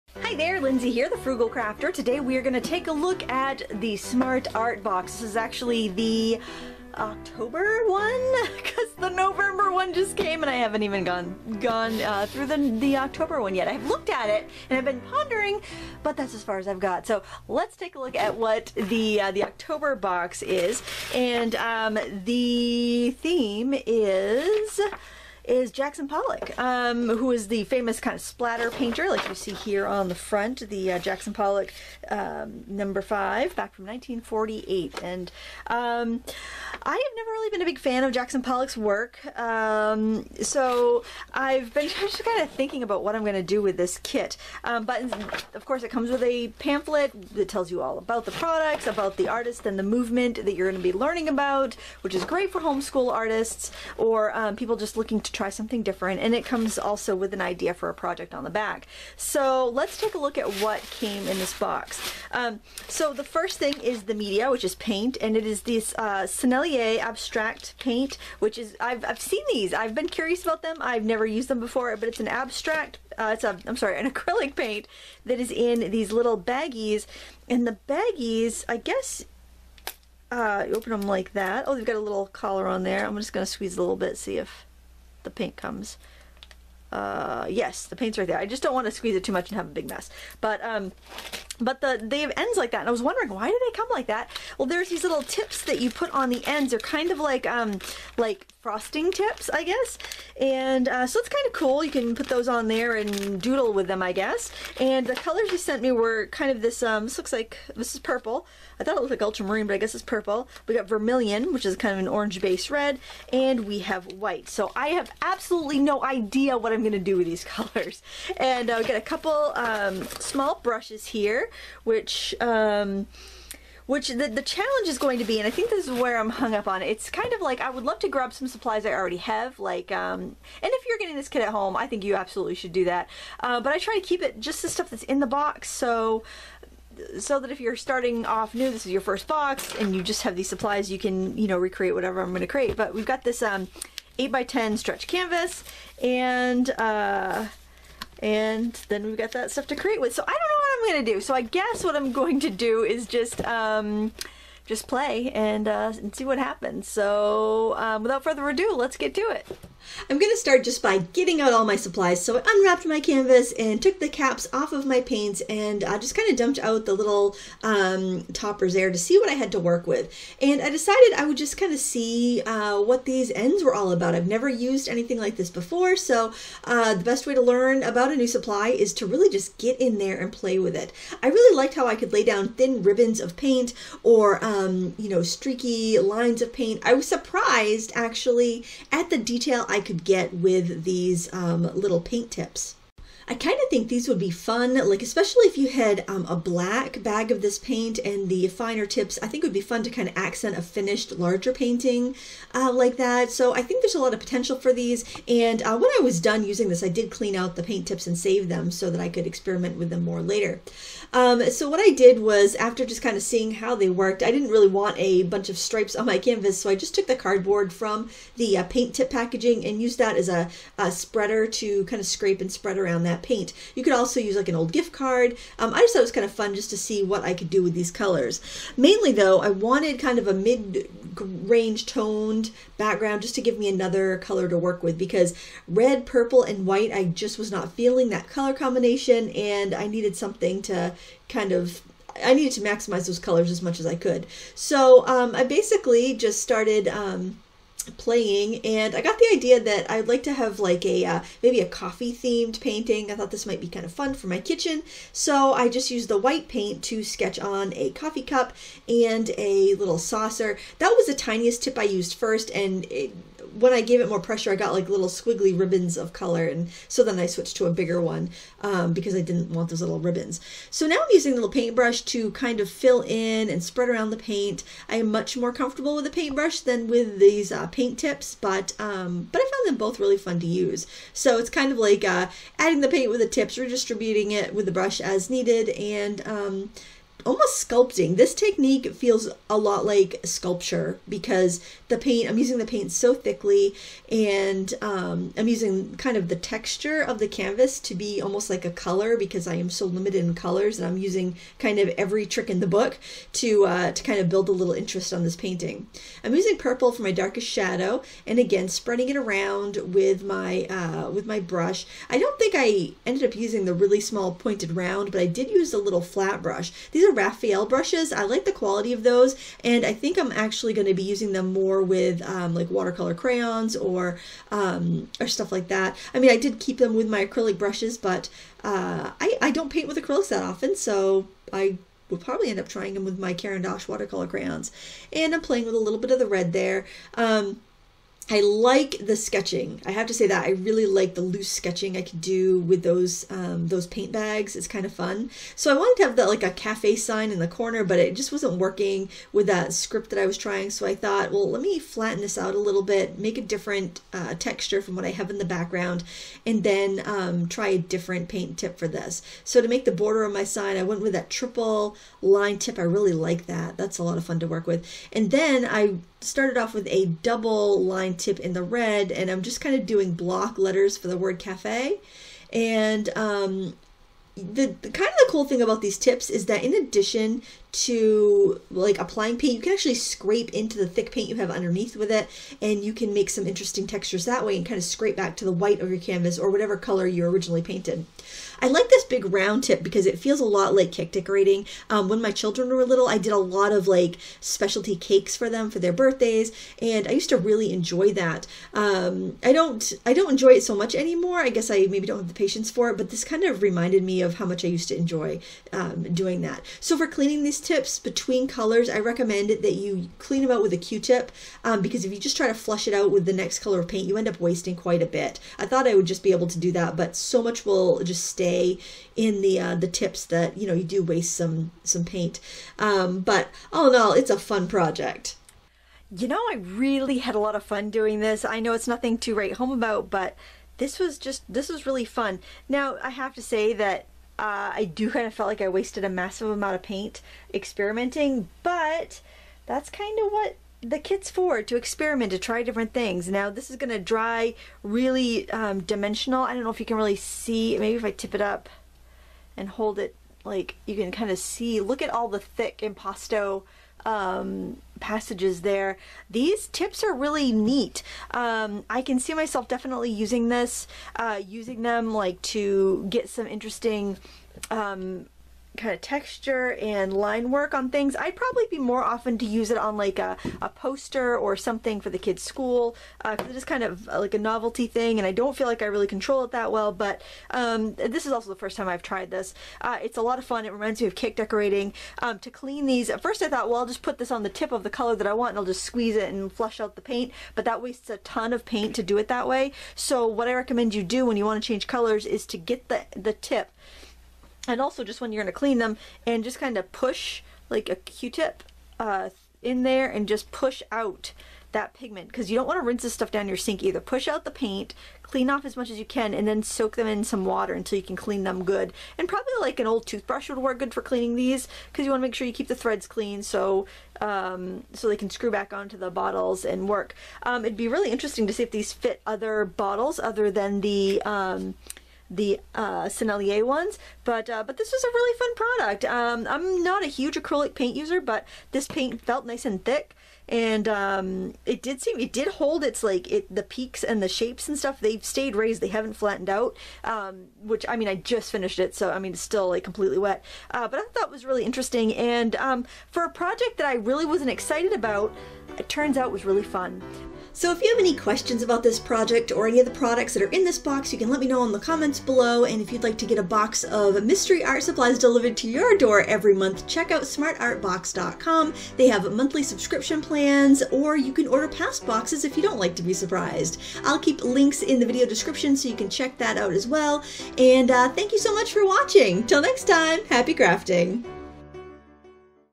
Impasto Acrylic Painting // Smart Art Box October 2017
All of the supplies I used were included in the October Smart Art Box
Sennelier Abstract Acrylic paints in Vermillion, Purple and White
Set of 8 painting tips
2 Raphael brushes
8"x10" stretched canvas

The Sennelier paint tips and bags of paint felt much like piping frosting, it was a lot of fun to experiment but very messy. I recommend putting your canvas on a scrap of cardboard while working. The tips can also be used to scrape paint away. This was a project I liked much more than I thought i would. Don't be shy, give it a try!

My goal is to provide you with inspiration! Want more inspiration, here are some classes!